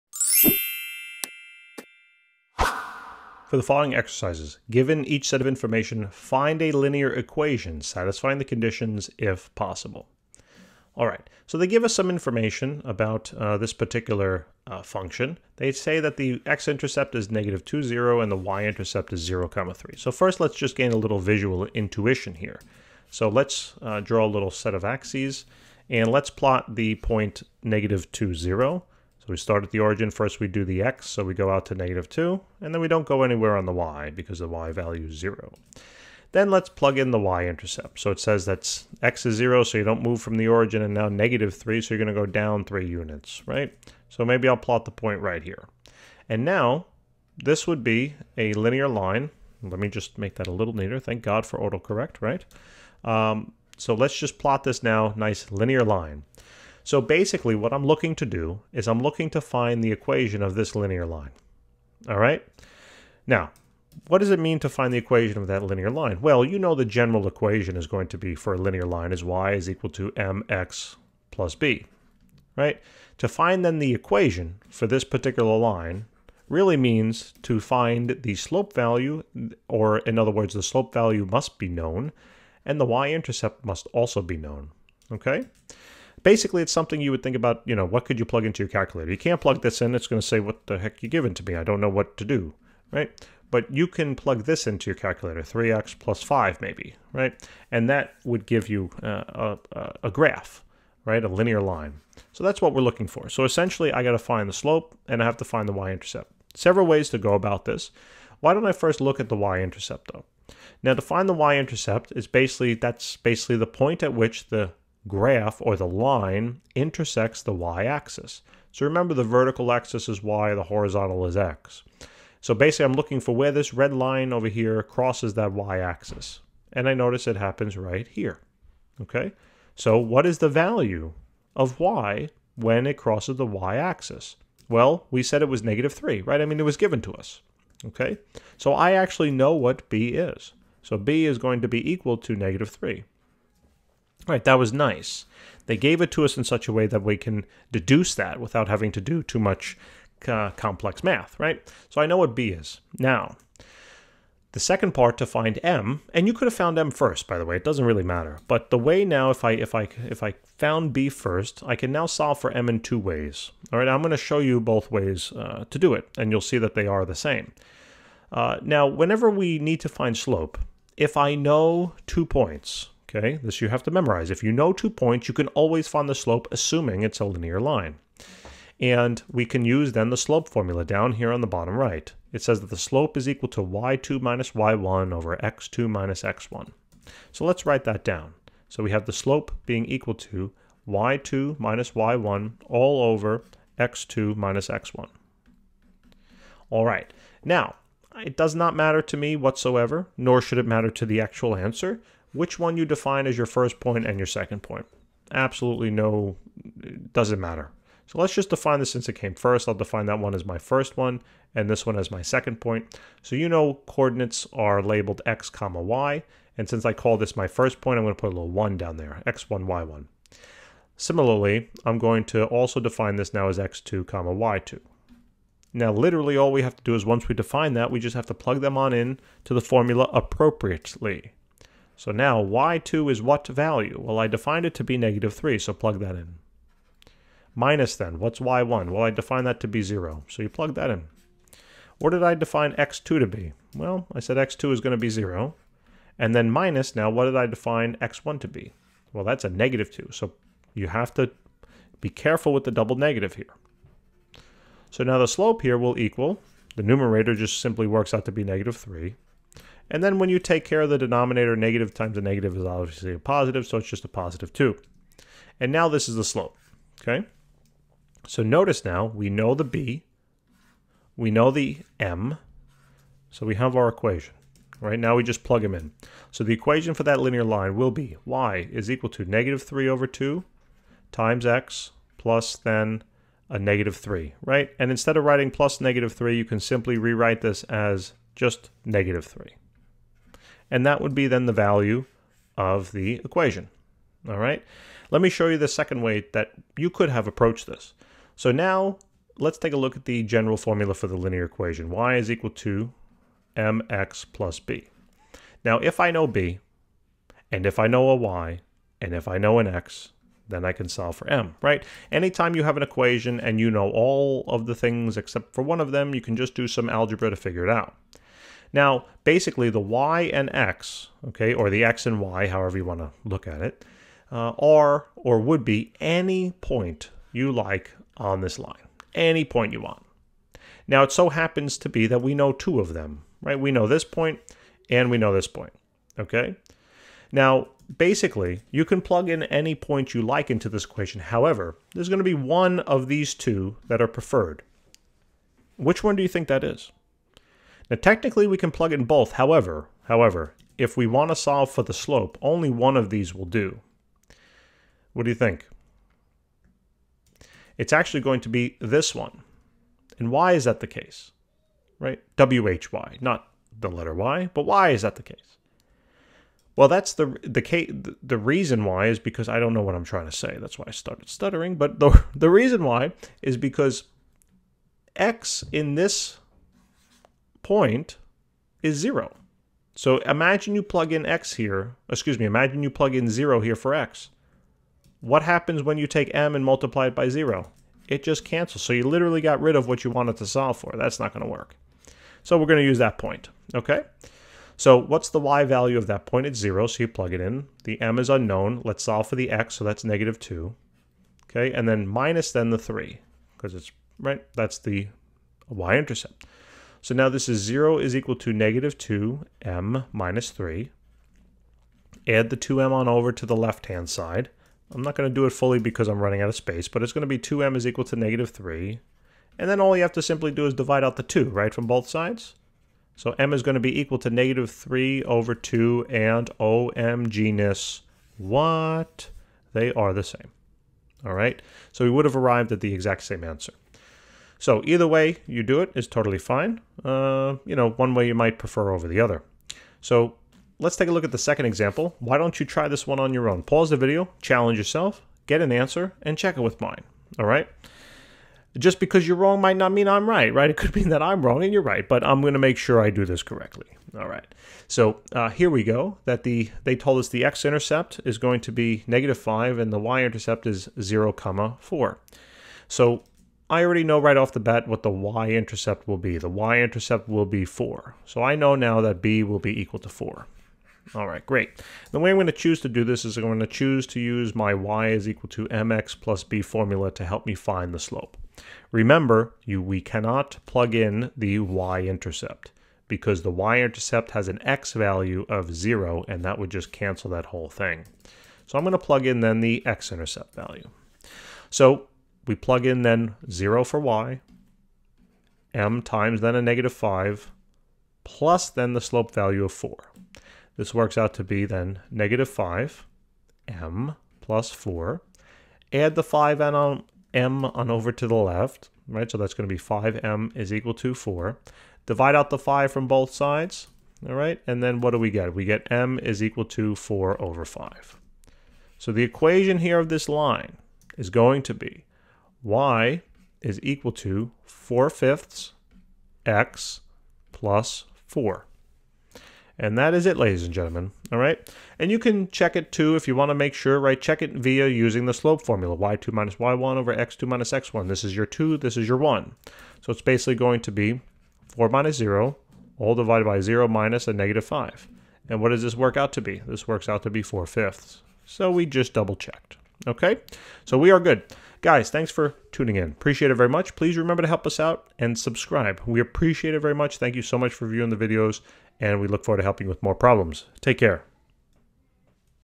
Finding a Linear Equation That Has an X-Intercept at (-2, 0) and a Y-Intercept at (0, -3)
For the following exercises, given each set of information, find a linear equation satisfying the conditions, if possible.
x-intercept at (−2, 0) and y-intercept at (0, −3)
x-intercept at (−5, 0) and y-intercept at (0, 4)